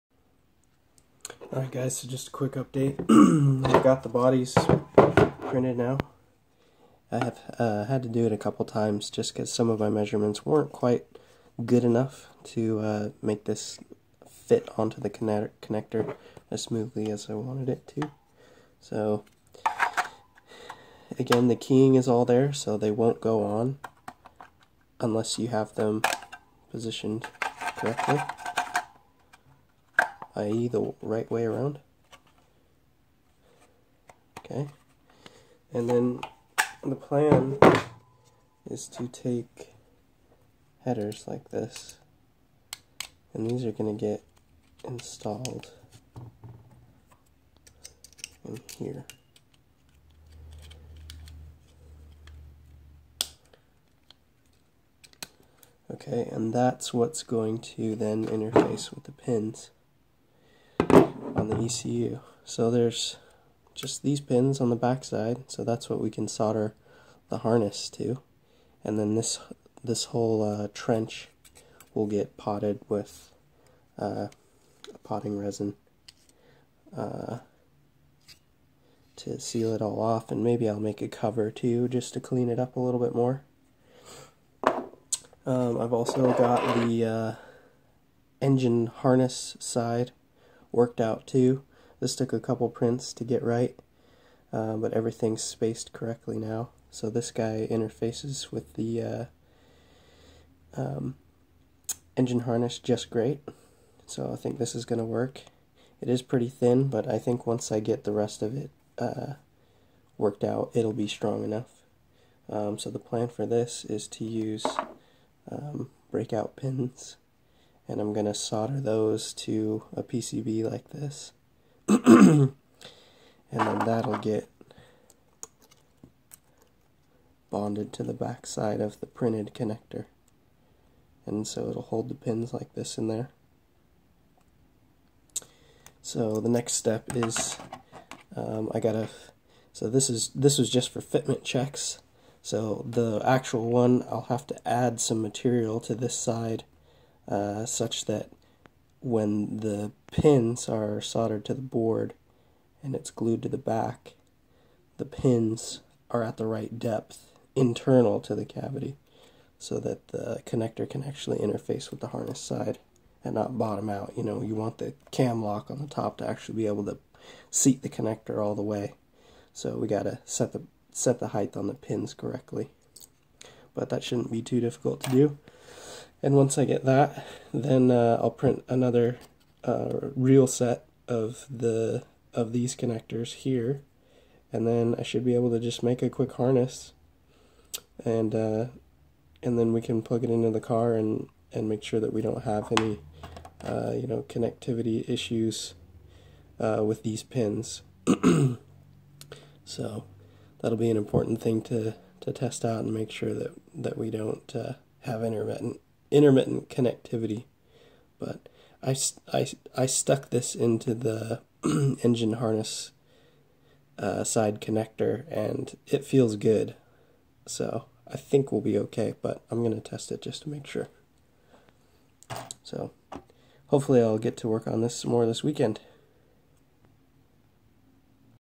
Gen Coupe 3.8 BK1 and BK2 3D Printed Harness Connectors (Part 2)
Going to try and make some connector housings that can be 3D printed in plastic. If this works, it would mean Genesis Coupe 3.8 BK1 and BK2 owners could fabricate a jumper harness so that other ECUs could be installed in the car and interface with the factory engine harness without having to buy an extra factory ECU to harvest the connectors from.